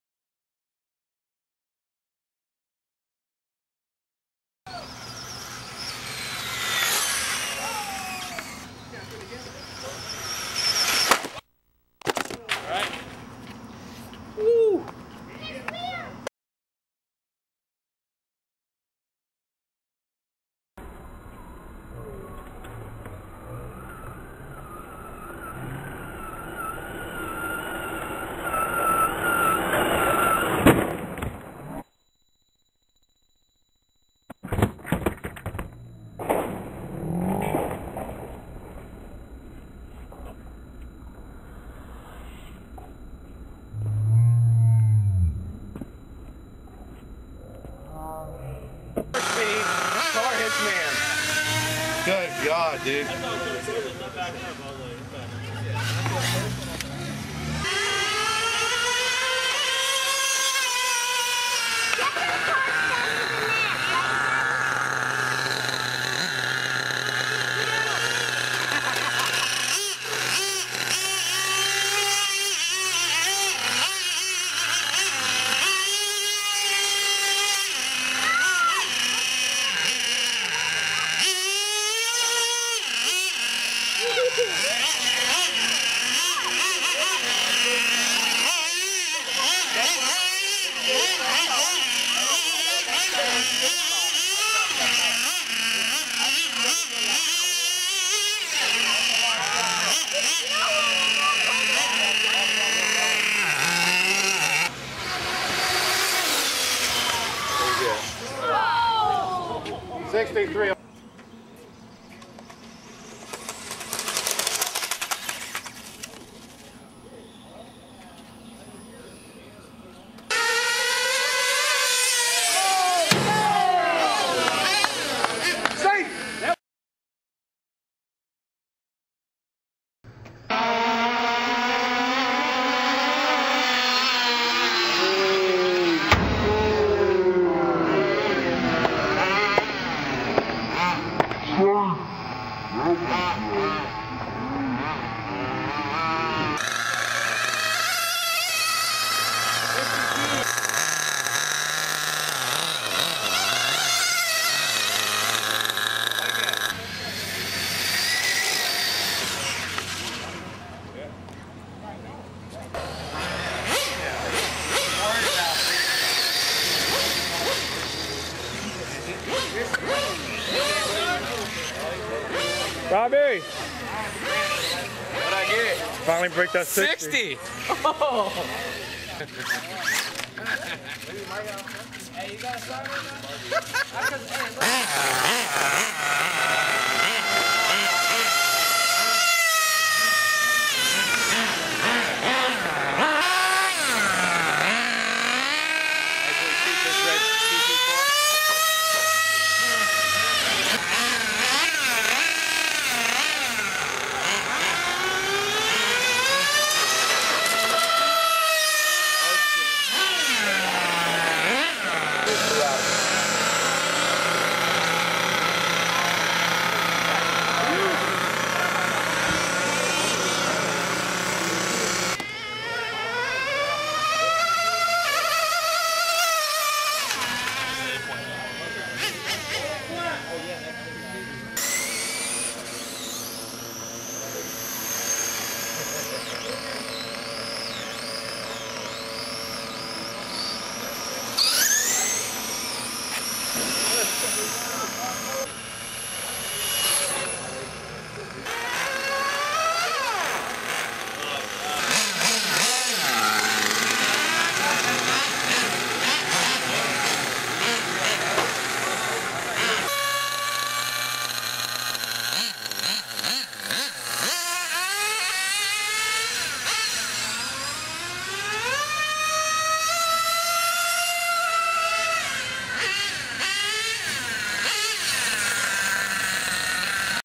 Traxxas Rustler VXL hits camera man
Got hit while filming a speed run. That was the first hit of the night. Later i got hit again but my buddy took the worst of it. The car was doing 70 plus and broke his foot and some how i got hit too. Too bad i wasn't filming then.